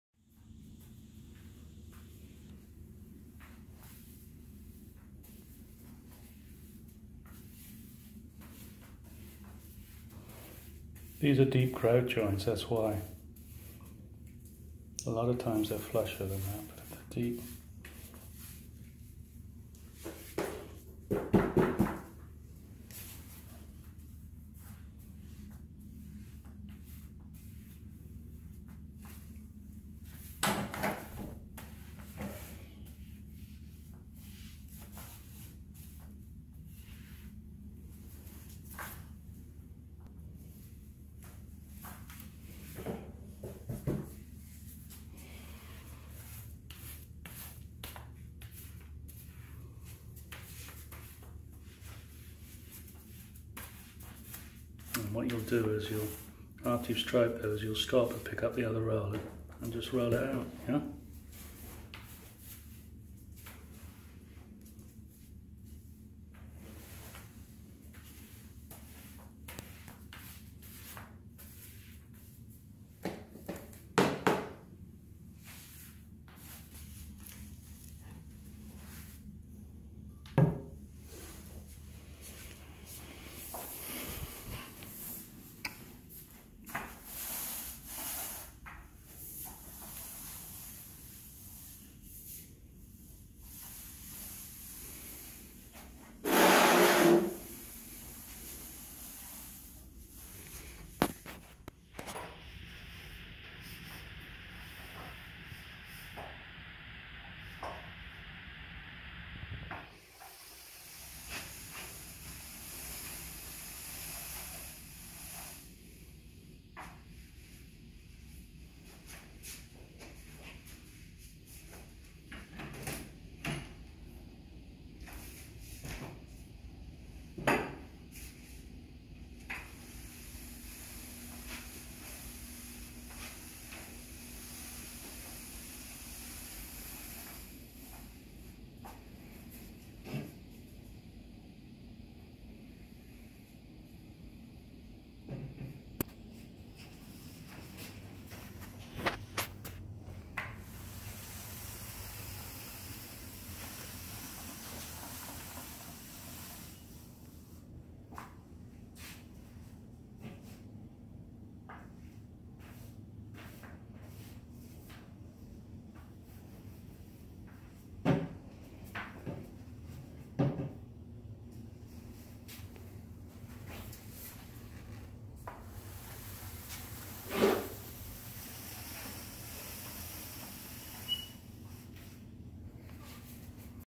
Sealing Deeper Grout Joints With GlazeGuard
Pre-striping grout joints with GlazeGuard using a mini roller